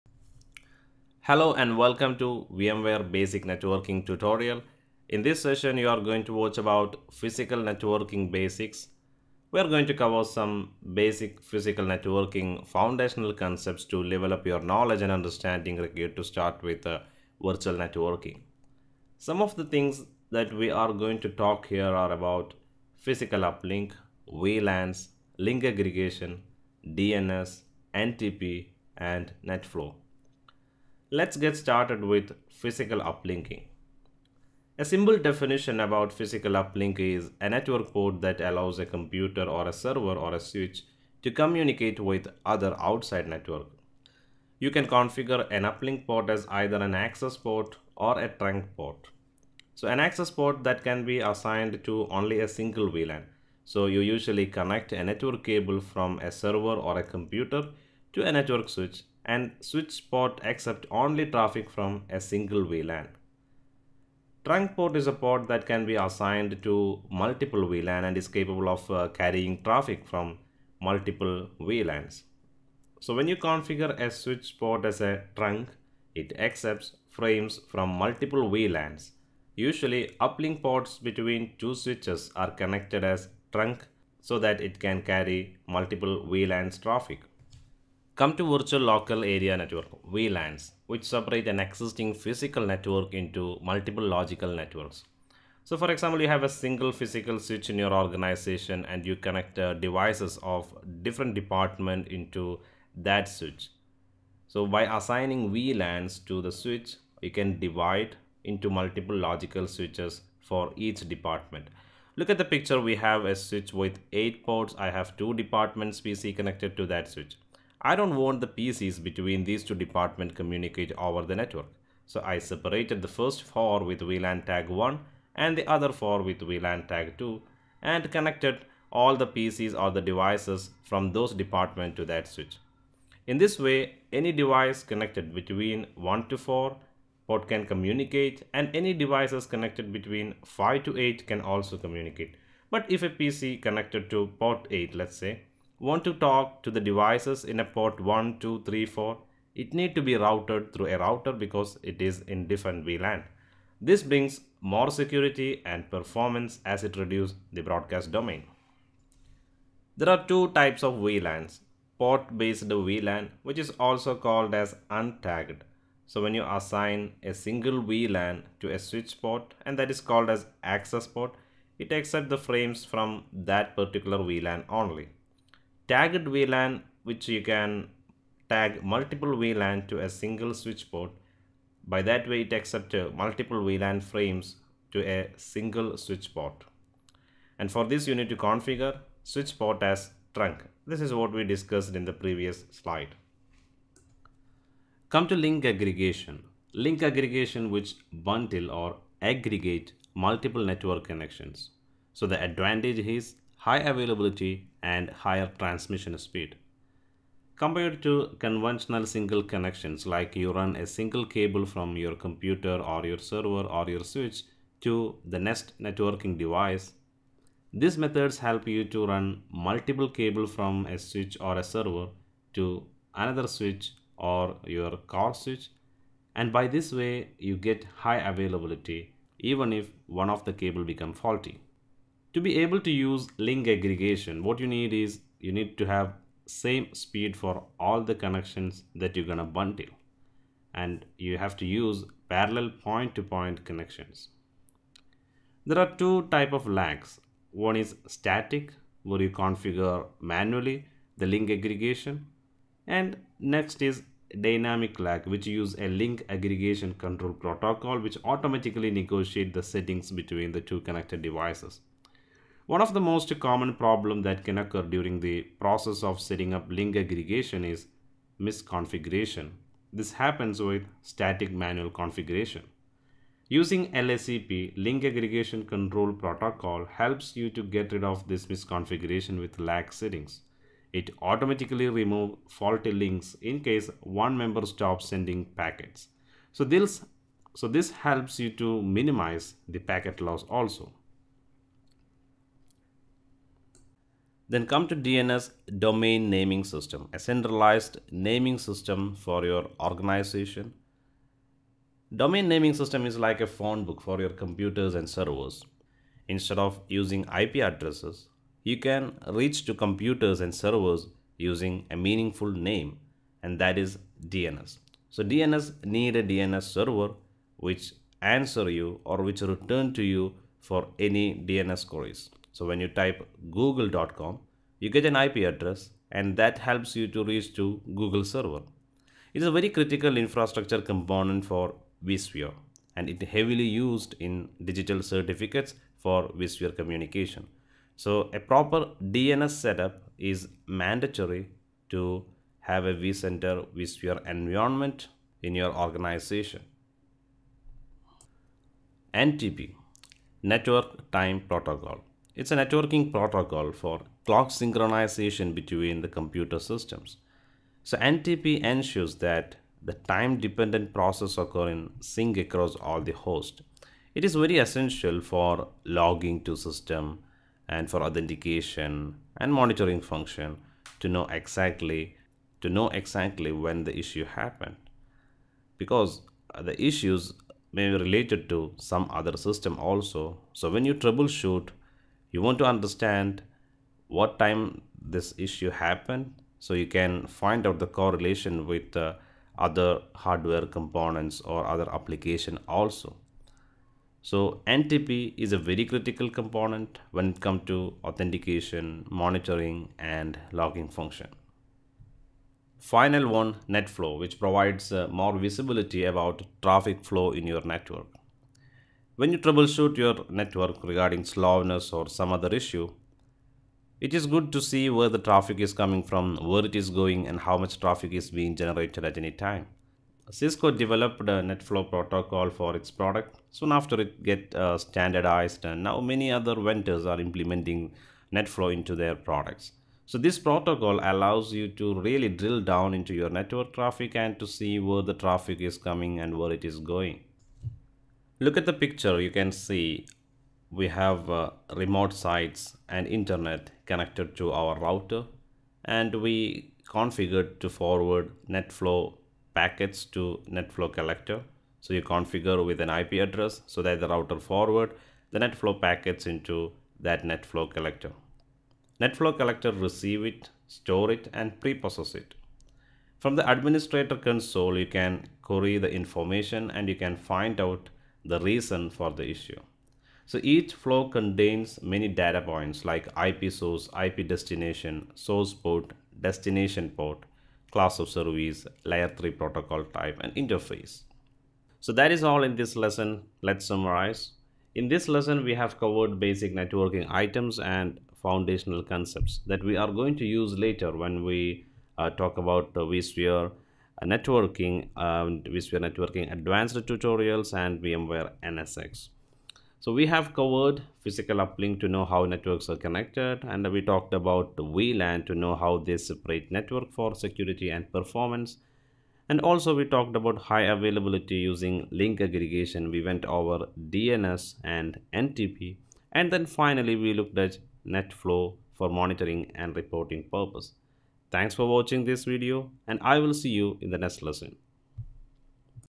VMware Basic Networking - Physical Networking Basics - Module 2 (Beginners)
This course is designed to teach the basics of networking in a VMware vSphere environment as well as give Demo for hands-on experience on installing and configuring the Virtual networks. By the end of this course, from a networking perspective, both virtual machine and host networking configuration will be covered including: Physical and Virtual Networking basics, Introduction to VMware, vSphere standard switch, vSphere distributed switch, Host and VM networking, Resource Management and Advanced Features.

In this lesion, We have covered basic networking foundational concepts required to level up your knowledge to start with virtual networking

Session6: vSphere Distributed switch